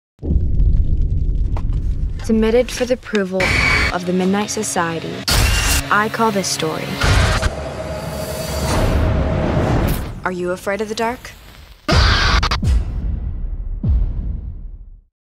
THE MIDNIGHT SOCIETY IS BACK!!!
Can the magic of the original 90's horror anthology be recreated for a new generation? Are You Afraid of The Dark? is one of my all-time favorite shows so I'm going to try and be open minded towards this one! The AYAOTD Revival miniseries premieres October 2019.

The cast:
Sam Ashe Arnold - Gavin
Miya Cech - Akiko
Tamara Smart - Louise
Jeremy Taylor - Graham
Lyliana Wray - Rachel

Rafael Casal - The Carnival of Doom’s ringmaster, Mr. Topha